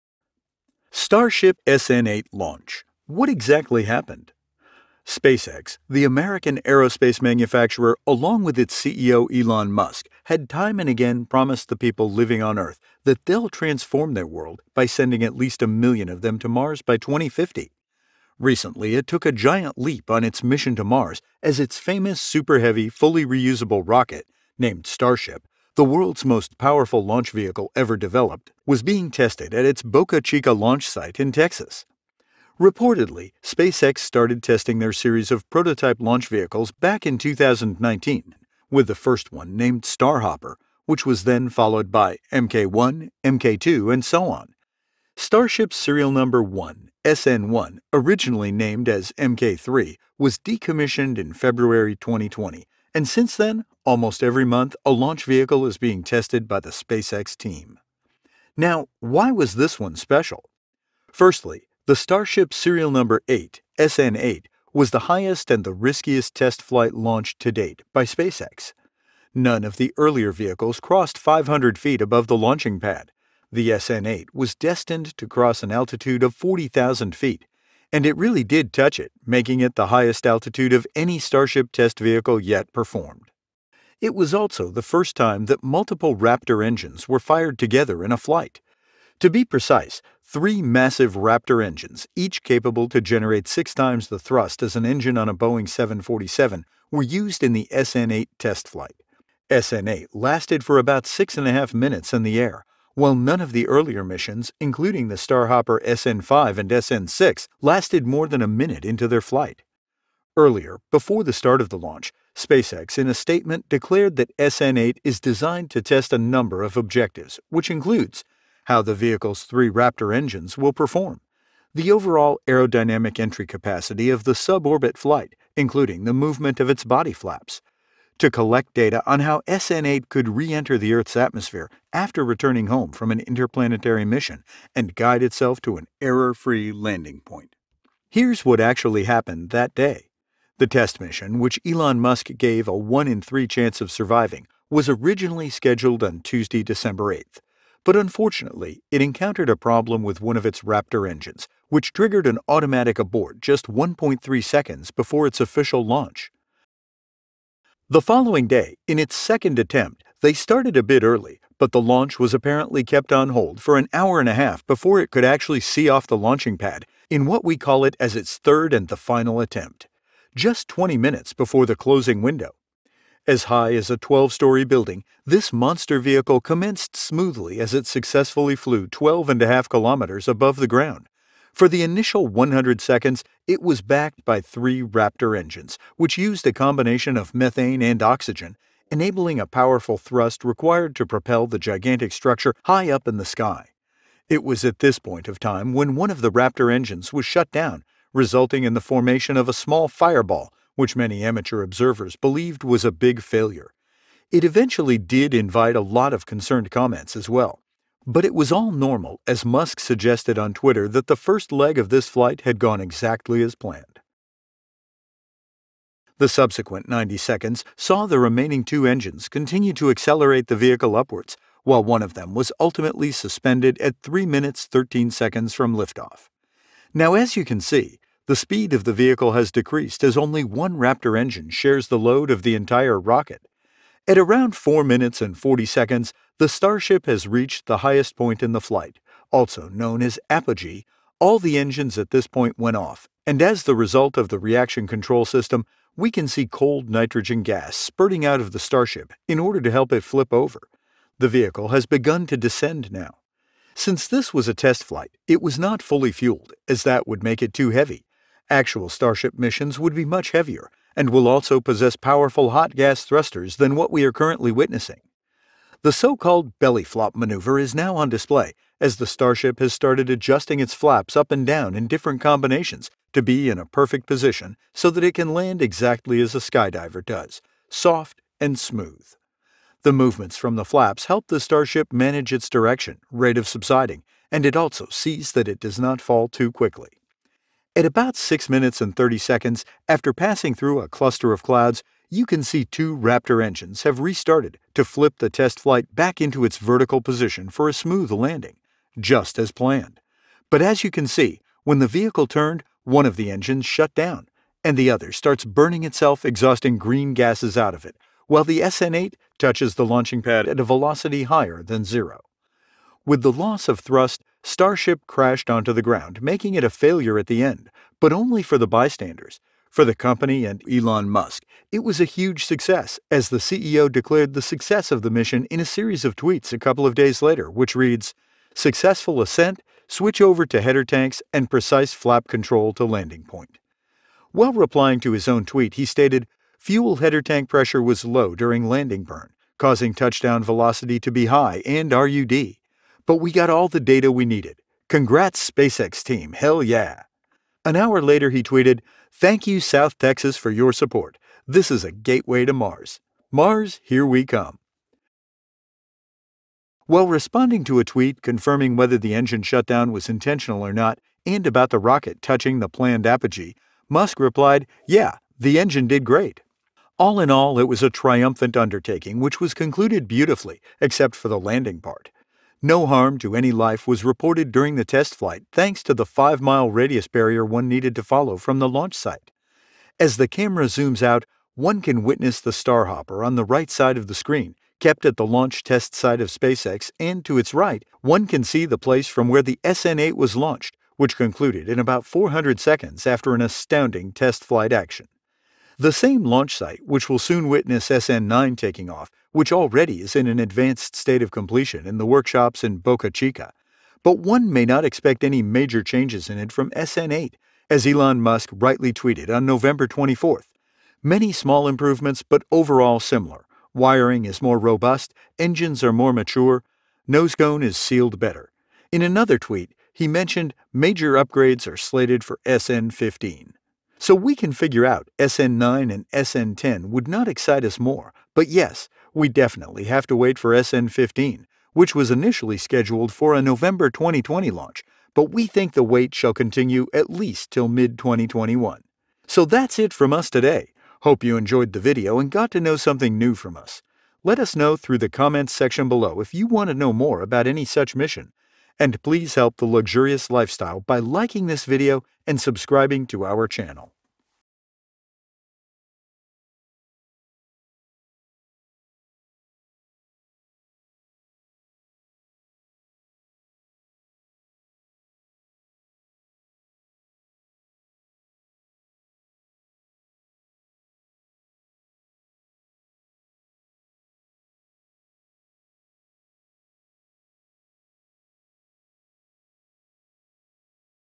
Starship SN8 Explosion - What Actually happened?
So in the the SN8 Launch and Subsequent Explosion, what actually happened?

SpaceX, the American aerospace manufacturer along with its CEO, Elon Musk had time and again promised the people living on Earth that they will transform their world by sending at least a 1,000,000 of them to Mars by 2050.

Recently, it took a giant leap on its ‘Mission to Mars’ as its famous super heavy fully reusable rocket named Starship, the world’s most powerful launch vehicle ever developed was being tested at its Boca Chica launch site in Texas.

Now, why was this Startshop SN8 "The Special One"?

- Firstly, the Starship Serial Number 8 (SN-8) was the highest and the riskiest test flight launch till date by SpaceX. None of the earlier vehicles crossed 500 feet above the launching pad. The SN-8 was destined to cross an altitude of forty thousand feet and it really did touch it, making it the highest altitude of any Starship test vehicle yet performed.

- It was also the first time, that multiple raptor engines were fired together in a flight. To be precise, three massive raptor engines each capable to generate six times the thrust as an engine on a Boeing 747 were used in the SN-8 test flight.

- SN-8 lasted for about six and a half minutes in the air while none of the earlier missions including the Starhopper, SN5, and SN6 lasted more than a minute into their flight.

In this video, we will try and explain what what actually happened on that day...